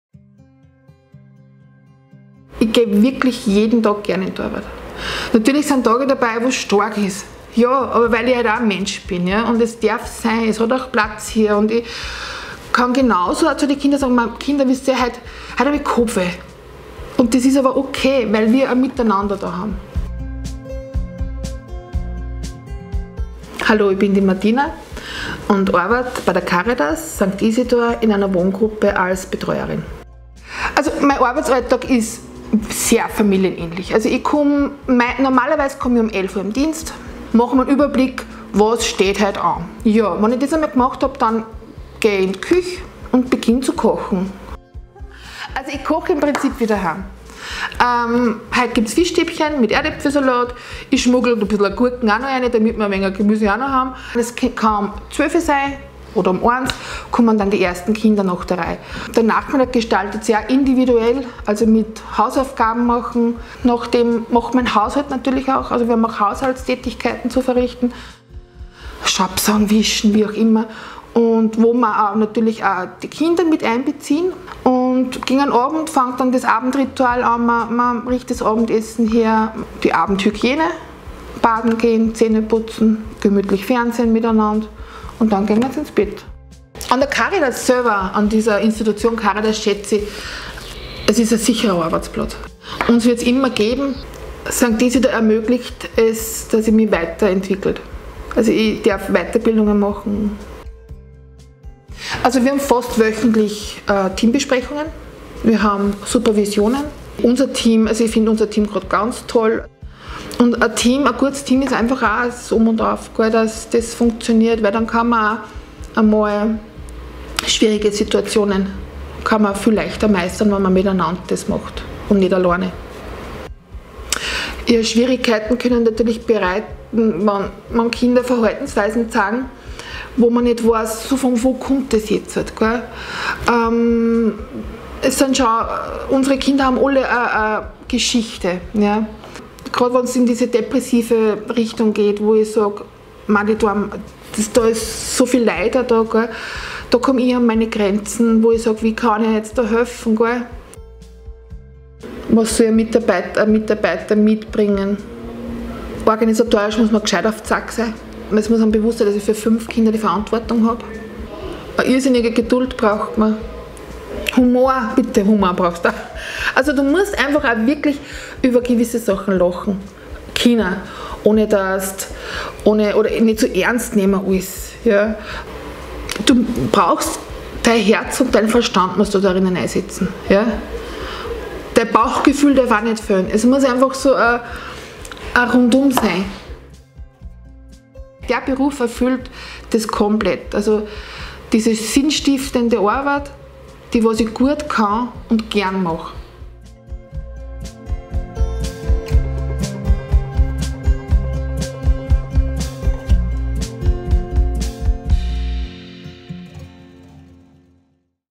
Martina Bauer Sozialbetreuerin in der Behindertenbegleitung, St. Isidor Caritas OÖ
Martina Bauer arbeitet seit über 20 Jahren als Sozialbetreuerin bei der Caritas OÖ in St. Isidor.
Mit 17 Jahren fing Martina hier als Überbrückung an - ihr Traumberuf war Hebamme. Doch als das mit dem Ausbildungsplatz zur Hebamme nicht klappte, ist sie geblieben. Heute ist sie froh darüber. Was Martina an ihrem Job liebt und warum sie gerne bei der Caritas arbeitet, erzählt sie in dem Video.

Komm auch du in unser starkes Team: jobs.caritas-ooe.at

Werde Teil der Caritas Community!